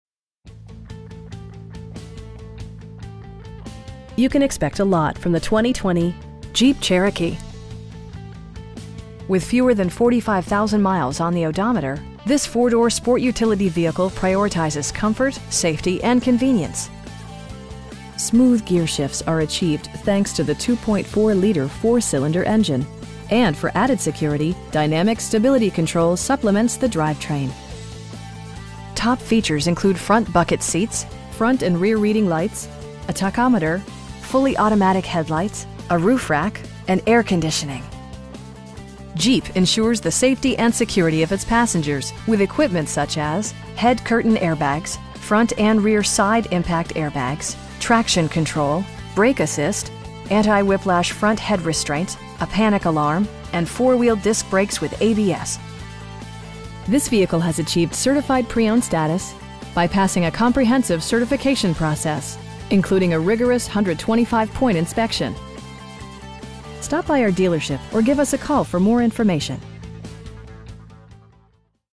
2020 Jeep Cherokee Latitude in Metairie, LA 70002-5842
Bergeron Chrysler Dodge Jeep Ram
3525 Veterans Memorial Blvd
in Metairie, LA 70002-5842

You can expect a lot from the 2020 Jeep Cherokee. With fewer than 45,000 miles on the odometer, this 4 door sport utility vehicle prioritizes comfort, safety and convenience. Smooth gearshifts are achieved thanks to the 2.4 liter 4 cylinder engine, and for added security, dynamic Stability Control supplements the drivetrain. Top features include front bucket seats, front and rear reading lights, a tachometer, fully automatic headlights, power windows, cruise control, a roof rack, and air conditioning. Jeep ensures the safety and security of its passengers with equipment such as: head curtain airbags, front and rear side impact airbags, traction control, brake assist, anti-whiplash front head restraints, a panic alarm, and 4 wheel disc brakes with ABS. This vehicle has achieved Certified Pre-Owned status, by passing a comprehensive certification process, including a rigorous 125 point inspection! Our aim is to provide our customers with the best prices and service at all times. Stop by our dealership or give us a call for more information.